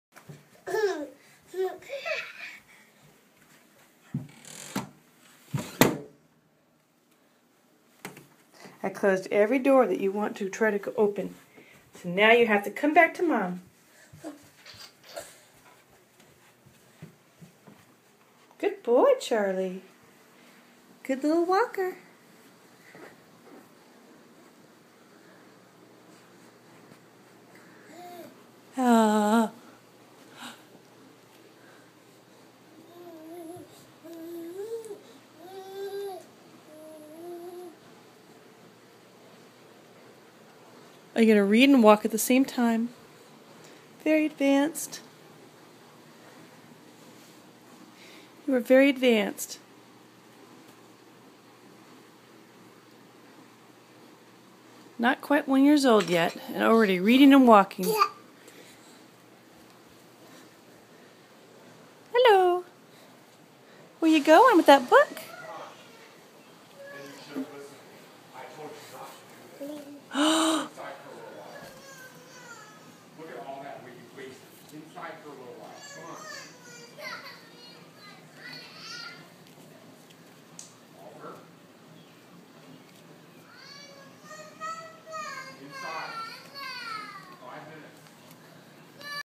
Read Walker
Can read & walk at the same time! 11months old.